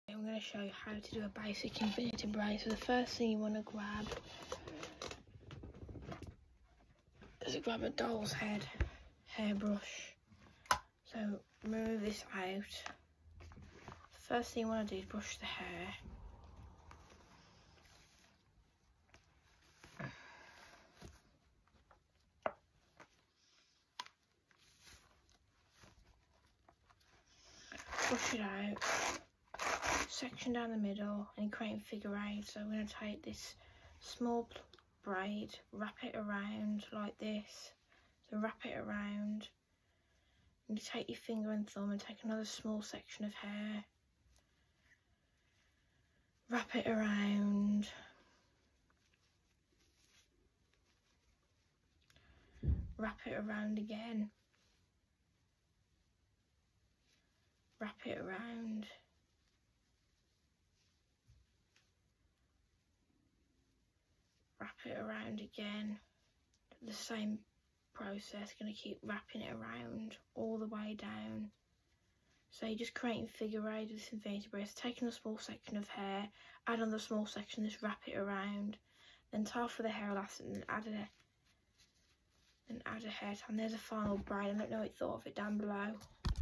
Basic infinity braid
Similar to a fishtail braid, take a small section of hair from the right side and cross that over the right section that is directly next to it. Next, cross the hair under the left section and wrap it around and over the left section. Then take it under and around the right